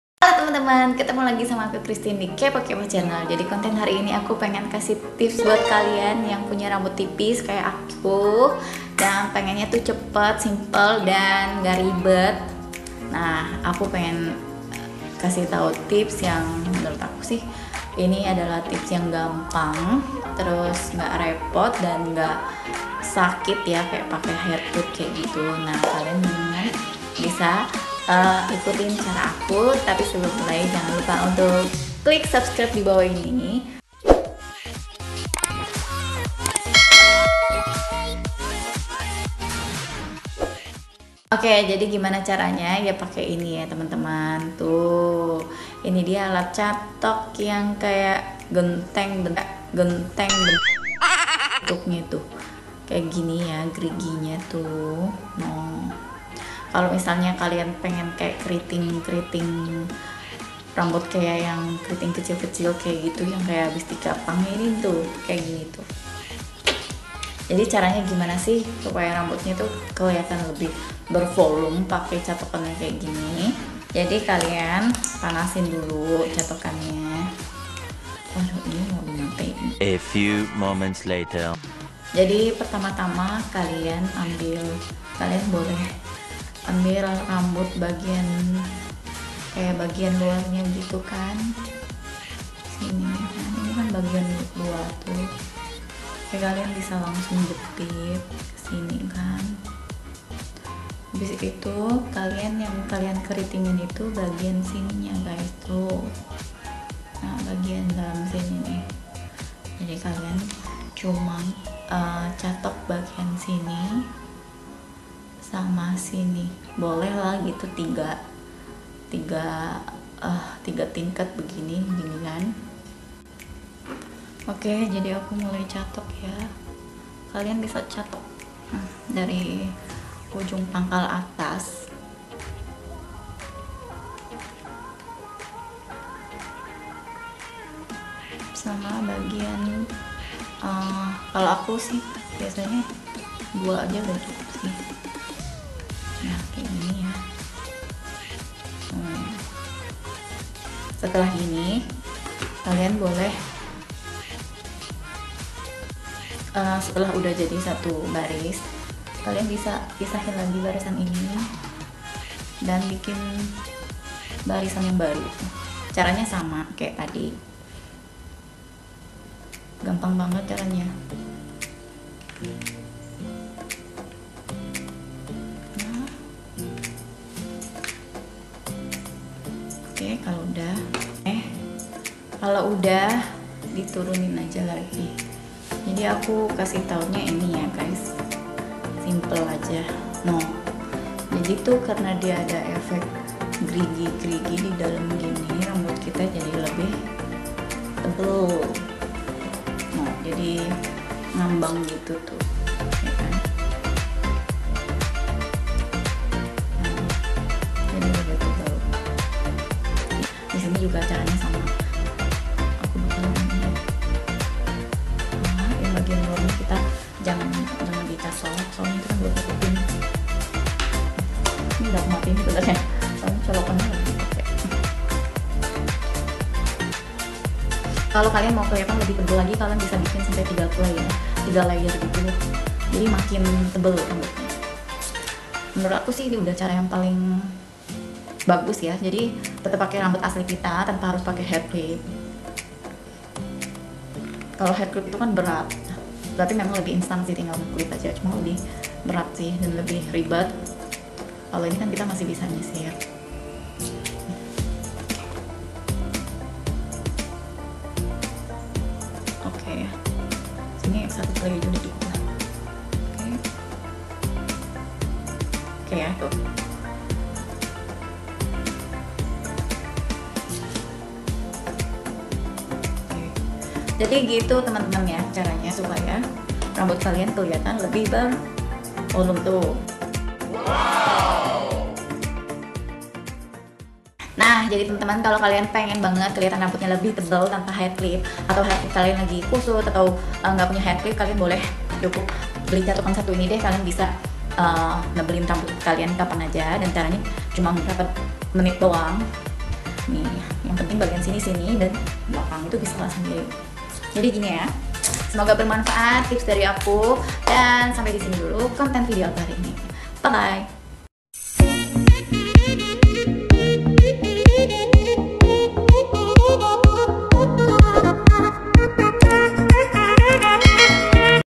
review catokan sonar gerigi menambah volume rambut
produk dishopee

atau kalian bs beli tokopedia atau dimana aja
merk lain juga bisa asal catokan gerigi namanya

thxyu